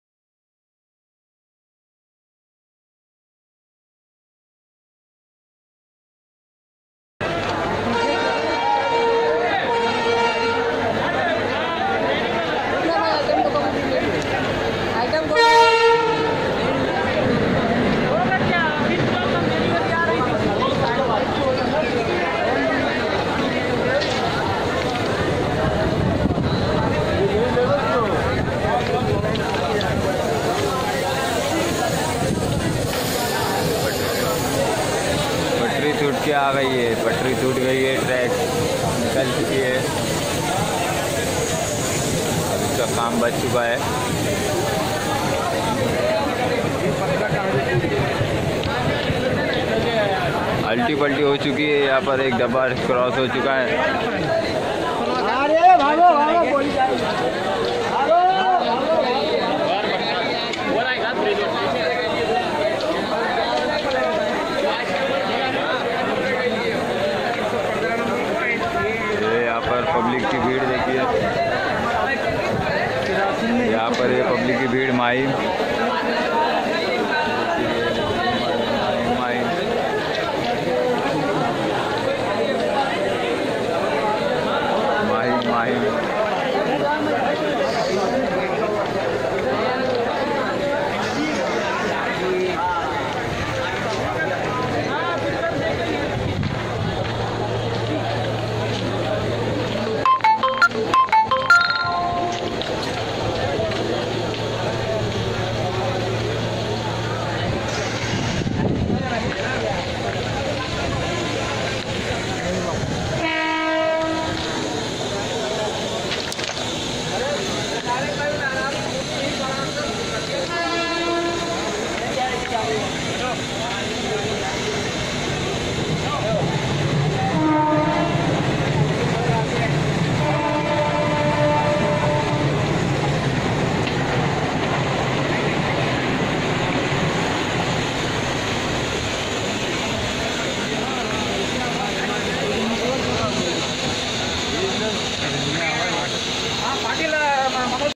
हार्बरमार्गावरील लोकल माहिमजवळ रूळावरून घसरली
आज सकाळी हार्बर मार्गावर लोकल ट्रेनचे चार डबे रुळावरून घसरले. यामुळे सीएसटीहुन अंधेरीकडे जाणारी लोकल वाहतूक ठप्प झाली. माहिमजवळ ही दुर्घटना घडली.